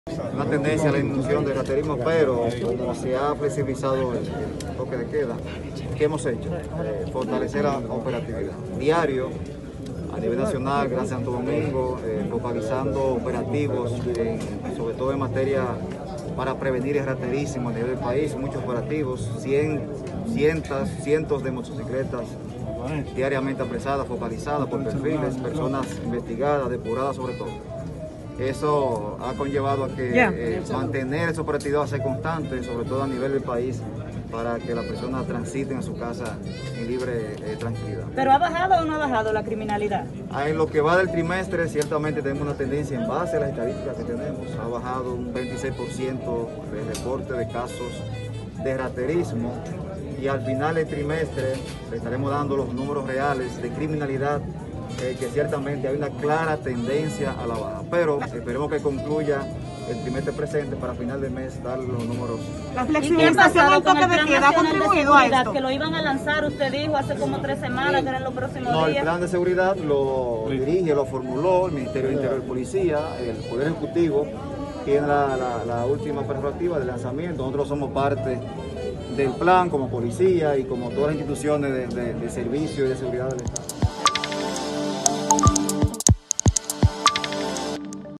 Director de la Policía asegura que hay una disminución del raterismo
Santo Domingo.- El director de la Policía Nacional, Edward Sánchez González, aseguró este lunes que el raterismo ha disminuido debido a una focalización de los operativos.